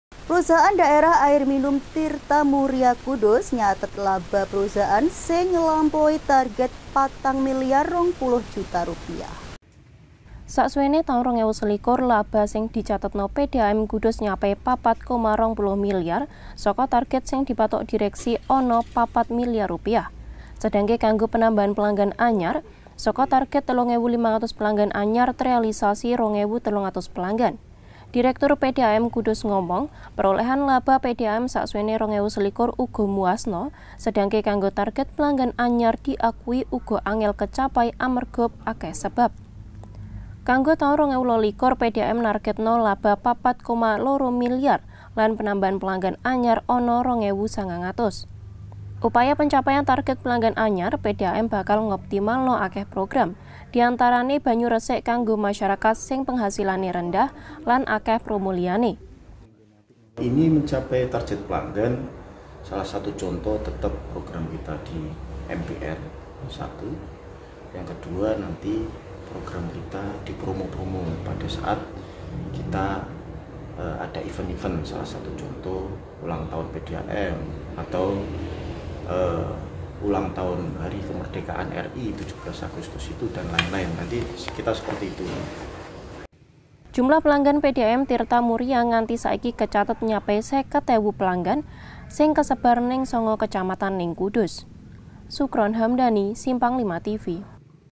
TARGET PELANGGAN ANYAR PDAM KUDUS DURUNG KECAPAI
PERUSAHAAN DAERAH AIR MINUM TIRTA MURIA KUDUS/ NYATET LABA PERUSAHAAN SENG NGELAMPAUI TARGET 4 MILYAR 20 JUTA RUPIAH